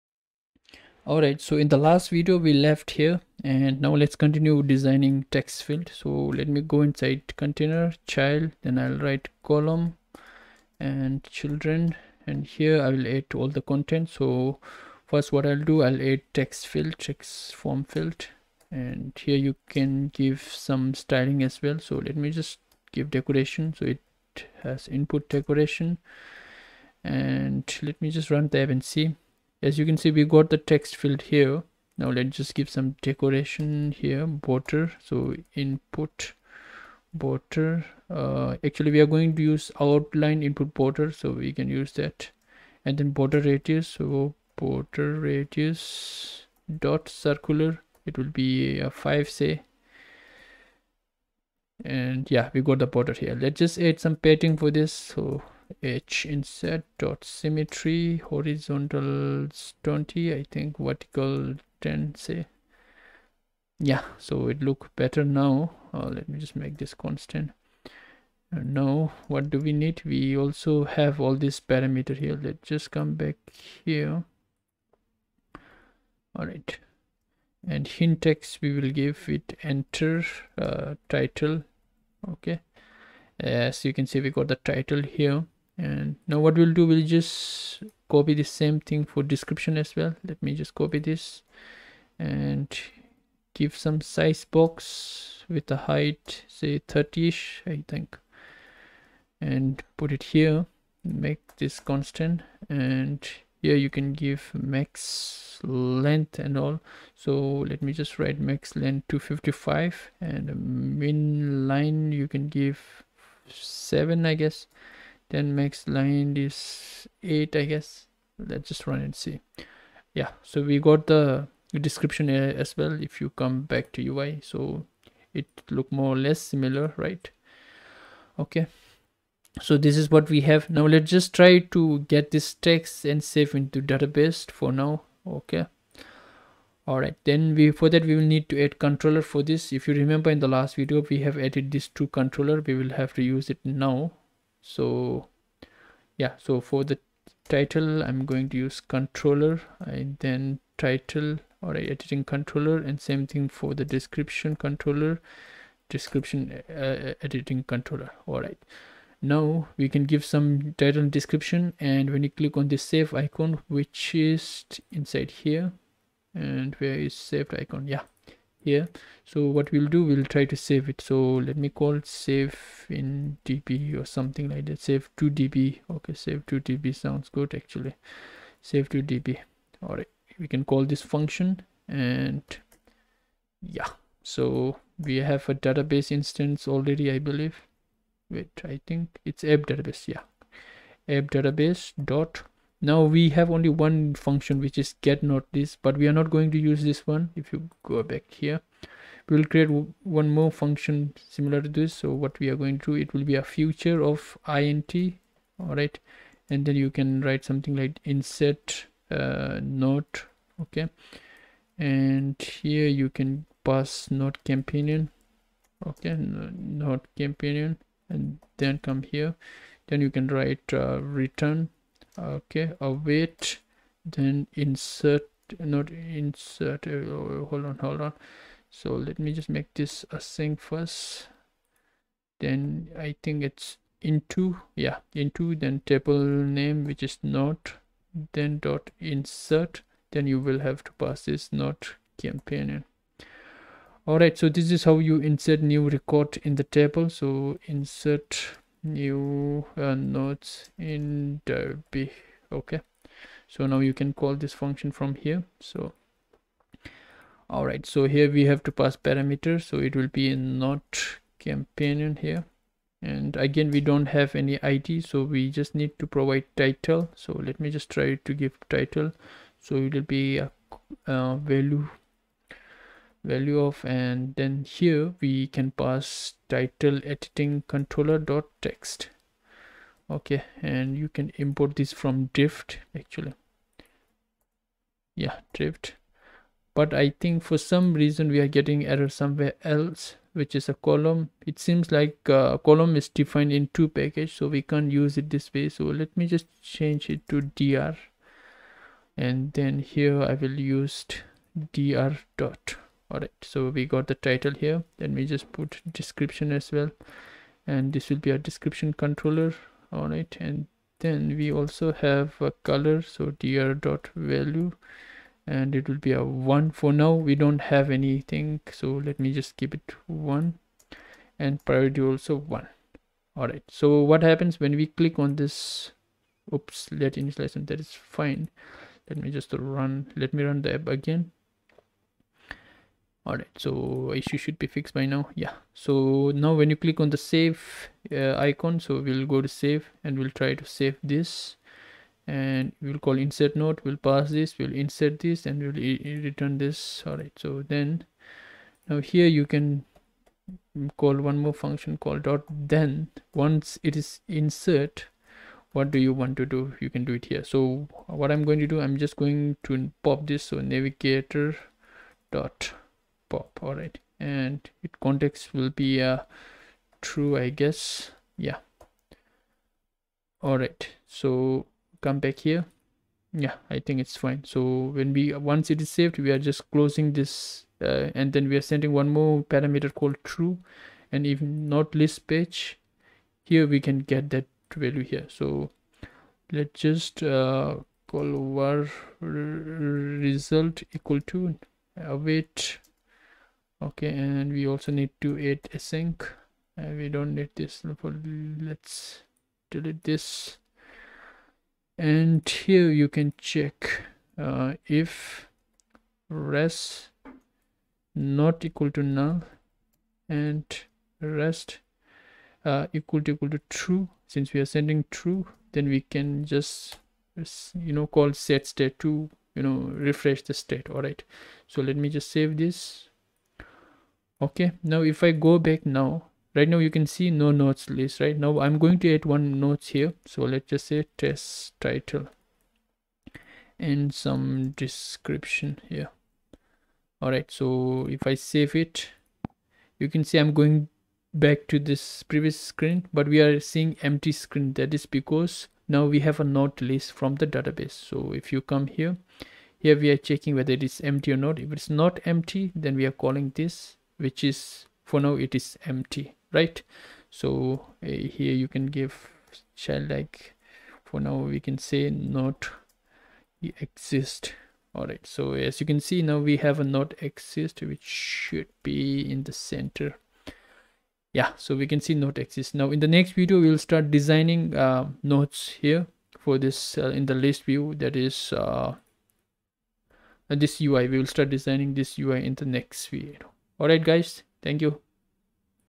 Flutter moor(drift) tutorial - Building NoteKeeper App from scrach | Saving Notes
[Source Code]
In this video we will discuss how to save in moor/drift database and base on that we can design UI as well.
Hi guys,
1. This project is inpired from Bimsina
2. I have posted a blog post on moor,check out in medium.